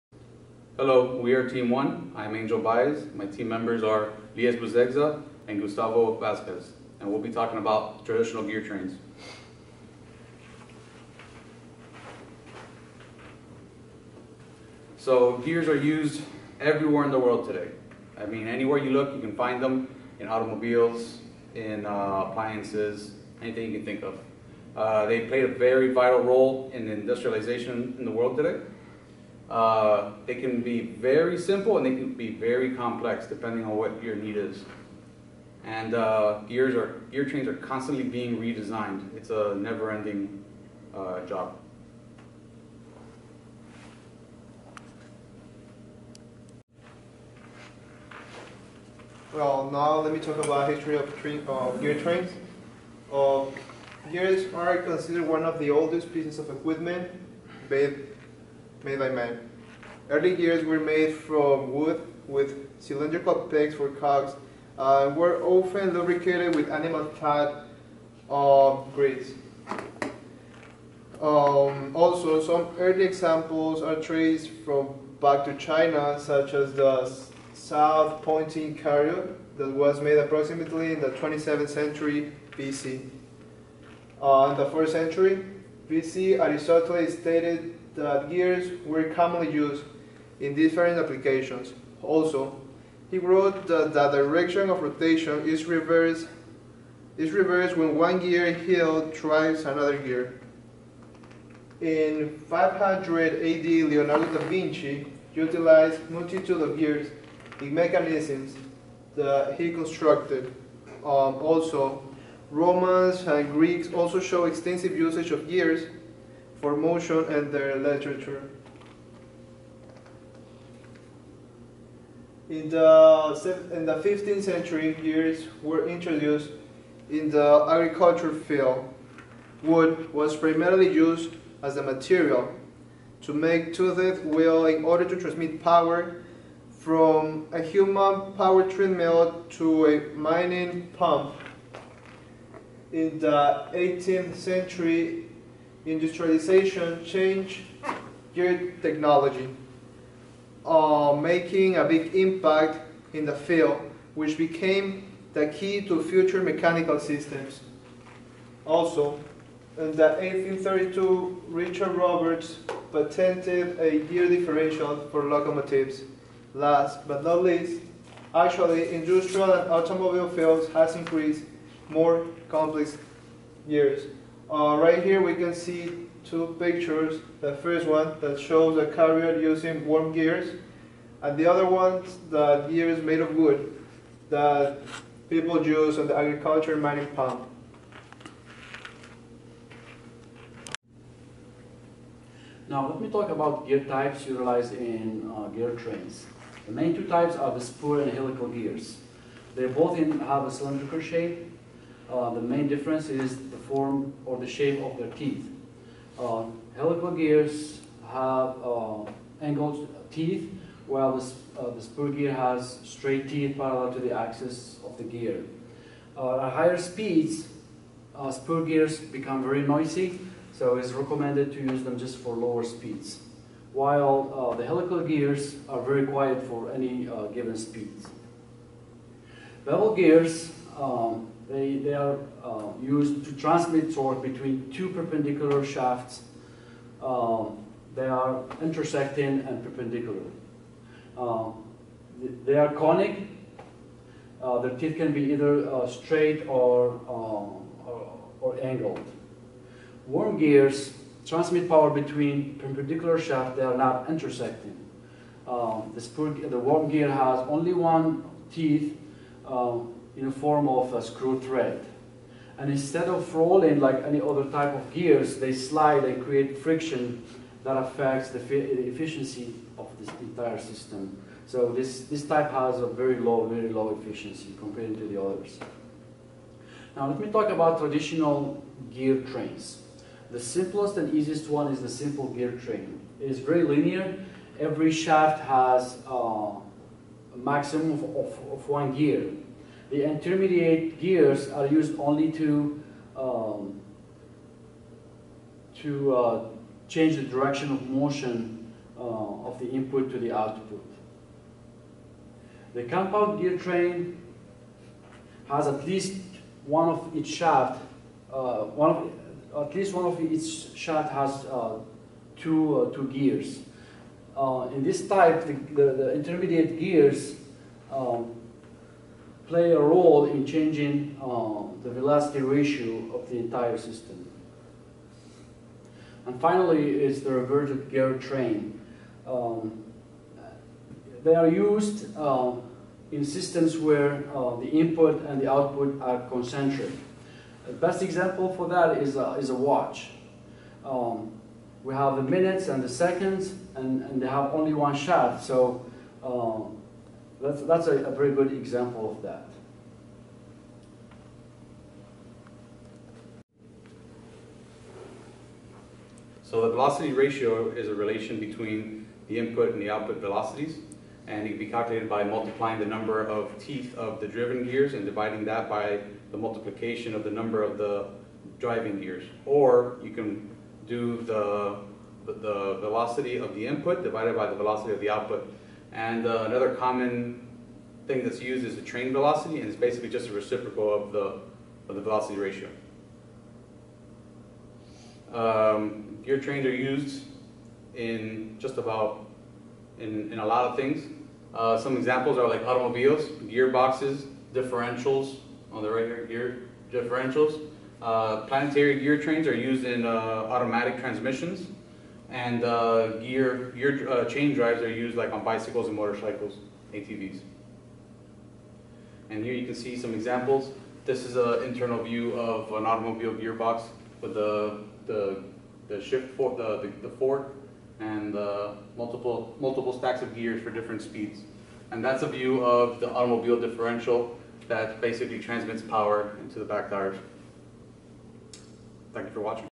Team 1: Gear Trains
Angel Baez, Lyes Bouzegza, Gustavo Vasquez

Gears have played a vital role in mans’ industrialization. Once developed to create very simplistic actions, gear trains have evolved into very complex systems over time.

Different forms of gears have been created and are utilized in different train configurations in order to accomplish specific tasks.  Just about everyone in the world utilizes some form of gear in one way or another, every single day, whether they know it or not.

Today’s economy is driven by transportation.  Without it, everything would come to a halt and struggle to move forward.  How does this pertain to the subject at hand you might ask?  Well, all forms of transportation equipment use numerous types of gear trains.

Gear design is a never ending job. Engineers are continuously improving their design in order to achieve better efficiency, higher strength, smaller packaging, and reduction of weight. This will ultimately open the doors to advancement in many other fields as well.

EML 4501 Mechanical Design II
Project Presentation
Department of Mechanical Engineering
10555 West Flagler Street
Florida International University
Miami, Florida 33174 USA

Professor Dr. Sabri Tosunoglu
TA: Neha Chawla